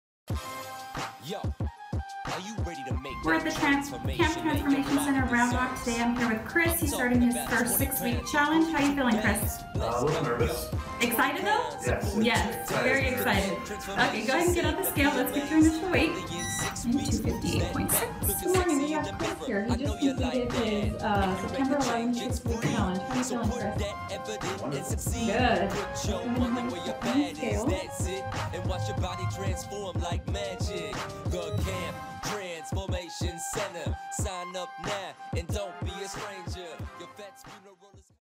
Round Rock TX Fitness 6 Week Challenge Results - Chris Kauffman
We will now check the result of Chris's 6 week weight loss challenge!